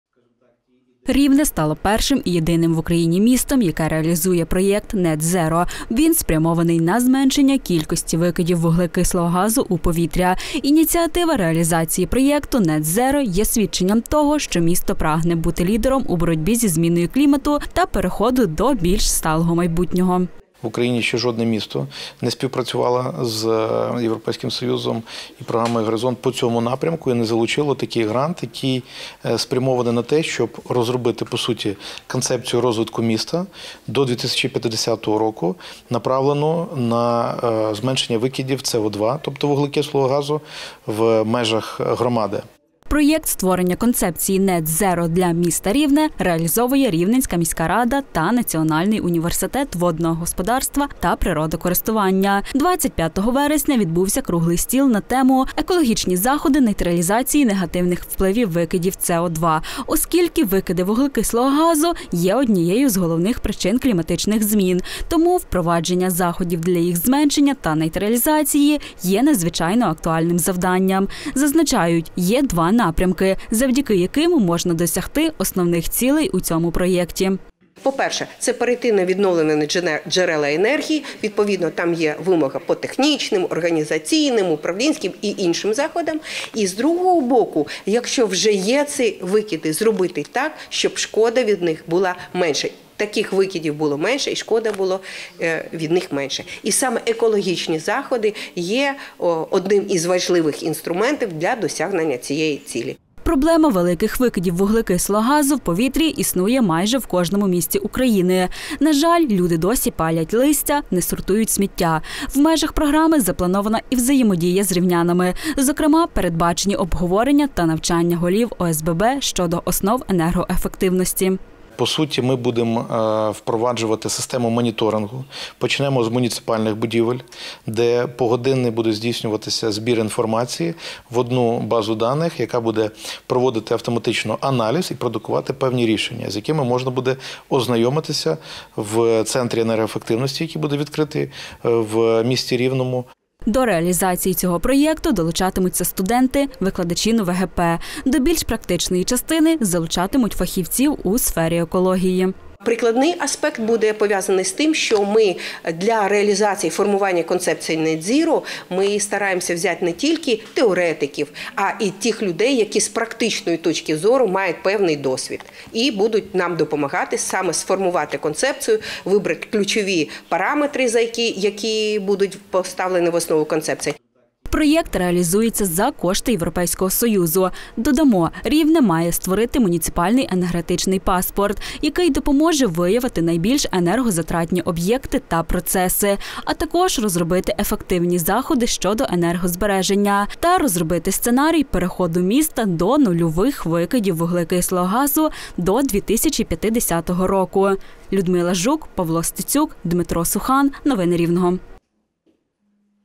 Зменшити викиди вуглекислого газу у повітря: у Рівному реалізовують проєкт NetZero
Єдине в Україні. Рівне стало першим містом в країні, яке виграло мільйон євро гранту на проєкт NetZero. В його рамках планують впровадити нові екологічні підходи, покращити транспорт, модернізувати будівлі, енергетичні системи та навести лад з відходами. Більше про проєкт та хто його реалізовуватиме - далі у сюжеті.